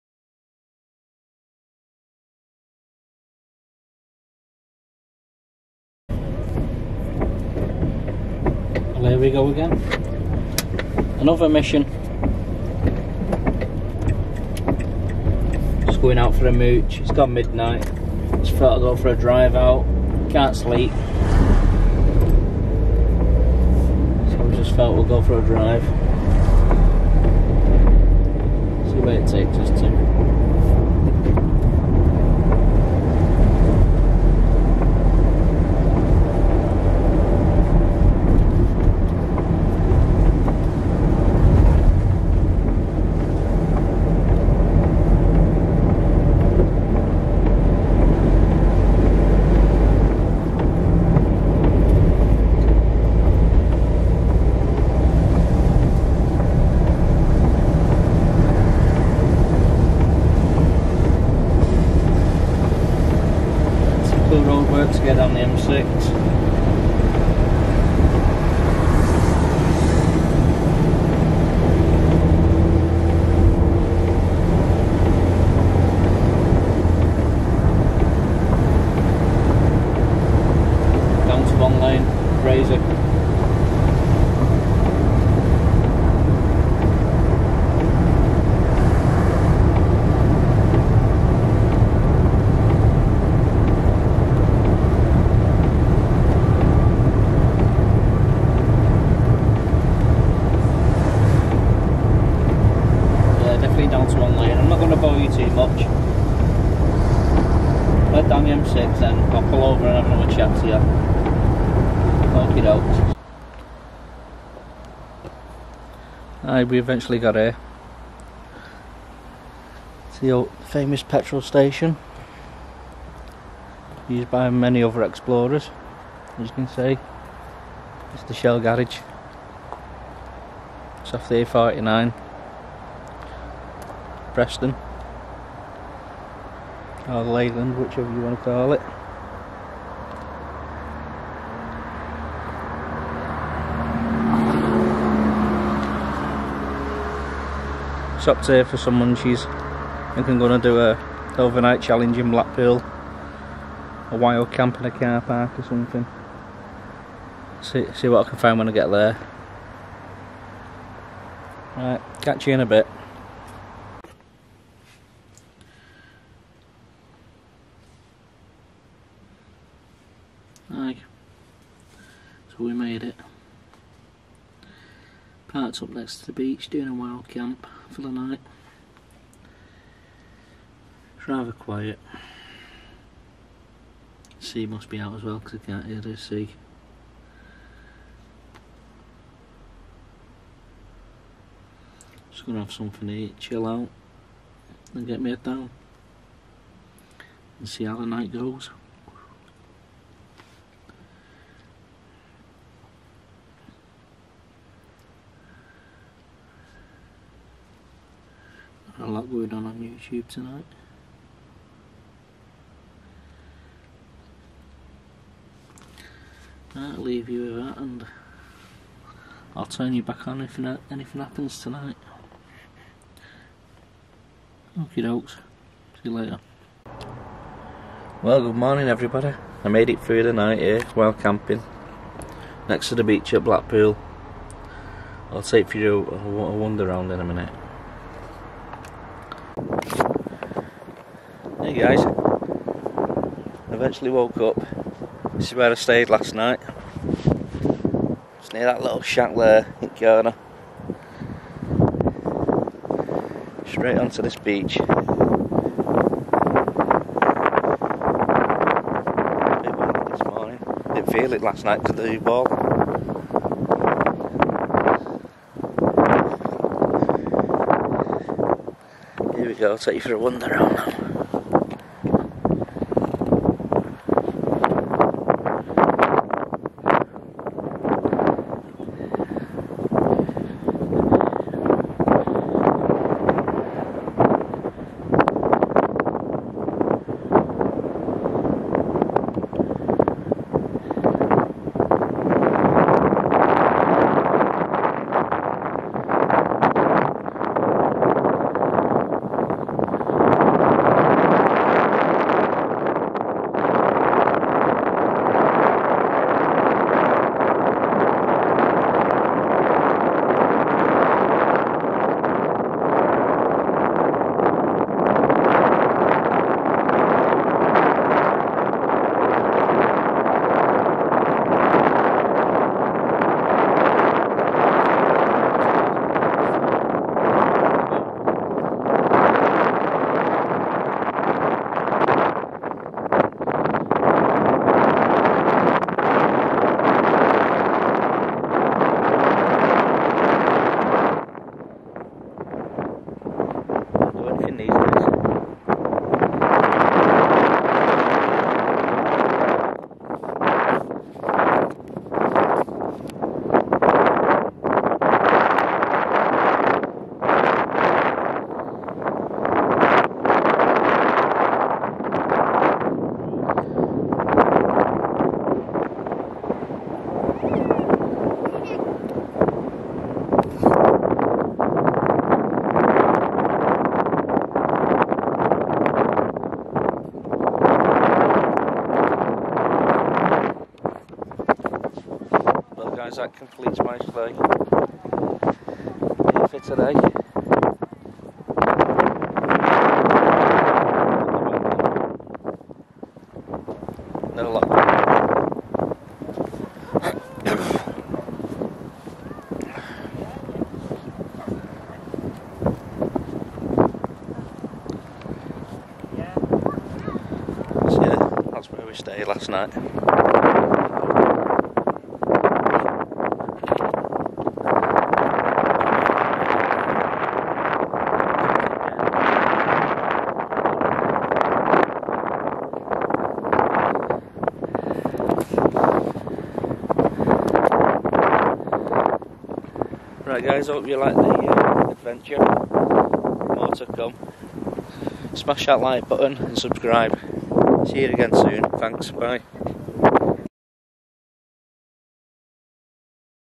BLACKPOOL WILD CAMPING WITH EXPLORING WITH CAMPERS
OVER NIGHT STAY IN BLACKPOOL NEXT TO BEACH